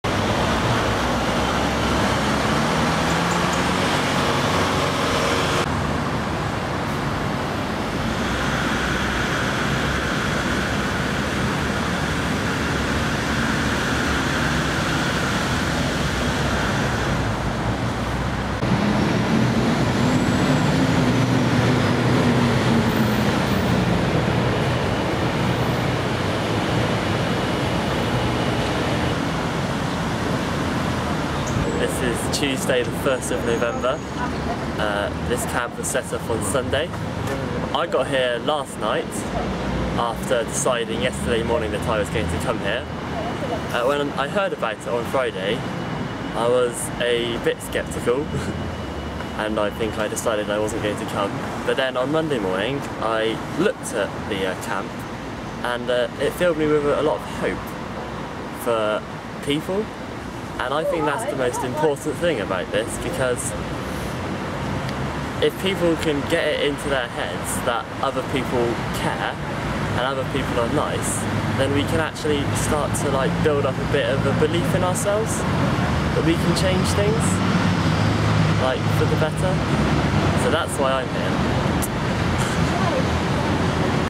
Occupy Bath #1
The Occupy movement graces the elegant Georgian Queen Square in the centre of Bath. Dissenters are the true patriots.